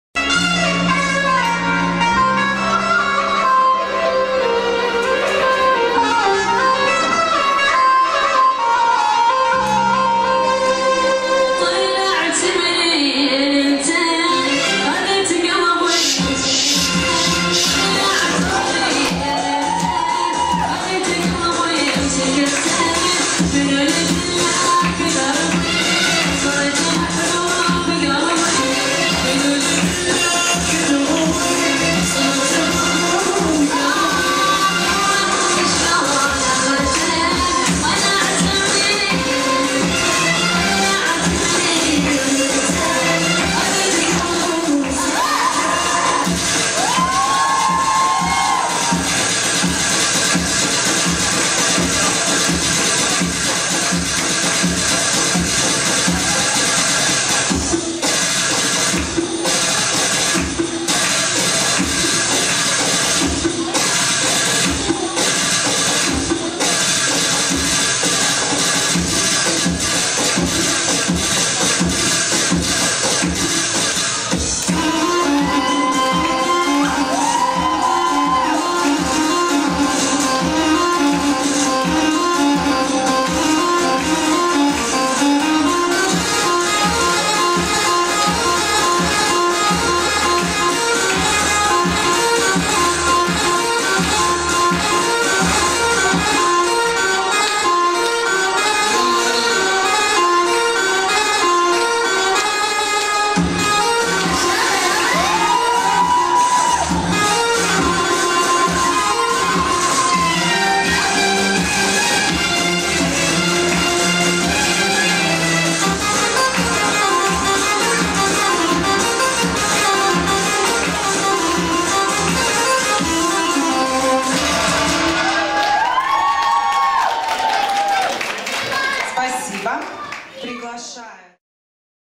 Oksana Sokolenko ⊰⊱ GLAMOUR bellydance fest '16.
Оксана Соколенко на конкурсе фестиваля «GLAMOUR bellydance 2016».
09.01.16. Киев. ул. Урицкого, 1А. Концерт-холл «Оазис»  (glamourfest.kiev.ua)
⊰⊰⊰⊰⊰    Фольклор Взрослые Продолжающие      ⊱⊱⊱⊱⊱